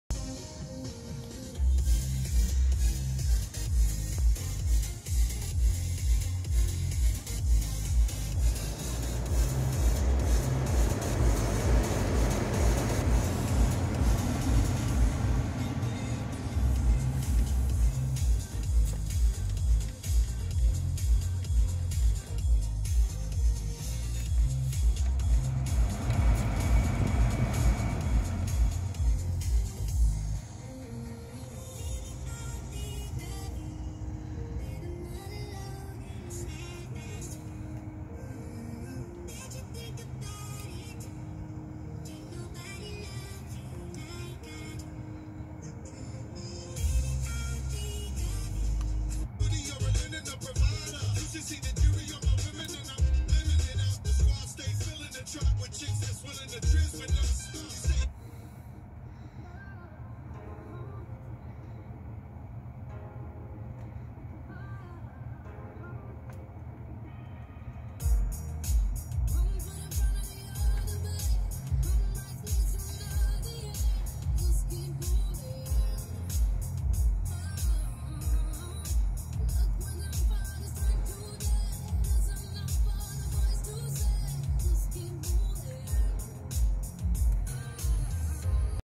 Belsee unit installed uk veloster head unit
The biggest pain is removing the surround to take the old unit and and put the new one in.